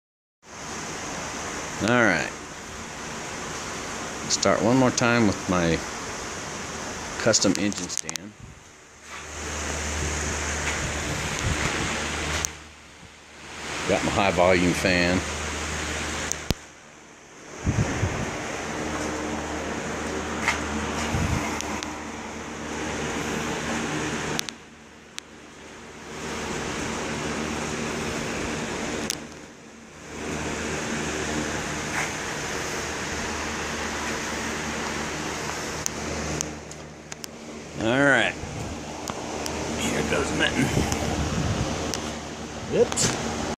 chevy 250 engine final start
one last start of the 250 after some tweeking. My "test stand" was pretty redneck, but it served the purpose! Motor runs awesome! Can't wait to drive this beast.